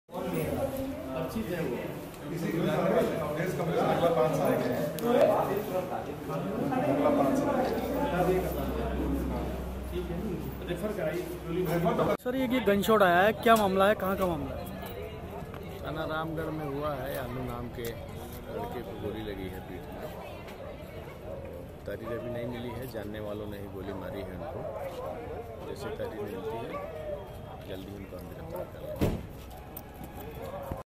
फिरोजाबाद : जरा सी बात पर अपनों ने ही चला दी ऑटो ड्राइवर पर गोली
फिरोजाबाद : प्रदेश में बीजेपी सरकार के लाख दावों के बावजूद अपराधों की संख्या में कोई कमी नहीं आ रही है. एक तरफ जहां लूटपाट और चोरियां लगातार हो रही हैं, वहीं दूसरी ओर हत्याओं की संख्याओं में भी कोई कमी नहीं है. ताजा मामला फिरोजाबाद का है.